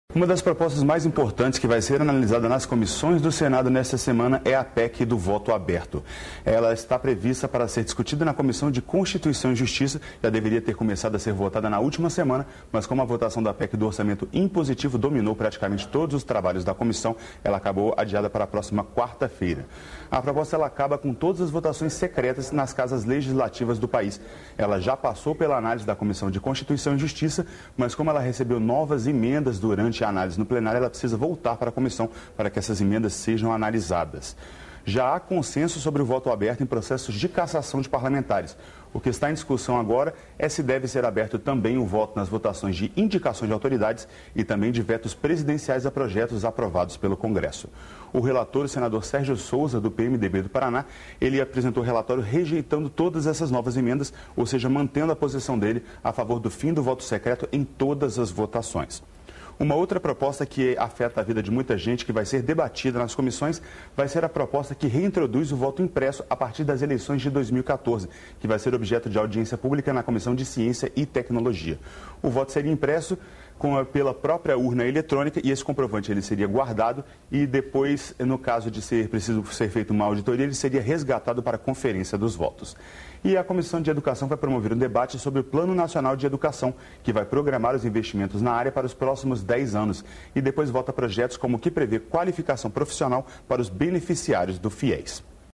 PEC do voto aberto é o destaque na pauta das comissões desta semana
A PEC que acaba com todas as votações secretas nas casas legislativas do país volta à Comissão de Constituição e Justiça para análise das emendas que foram apresentadas. Já a Comissão de Ciência e Tecnologia analisa a proposta que reintroduz o voto impresso a partir das eleições de 2014. As informações são do repórter Roberto Fragoso, da Rádio Senado.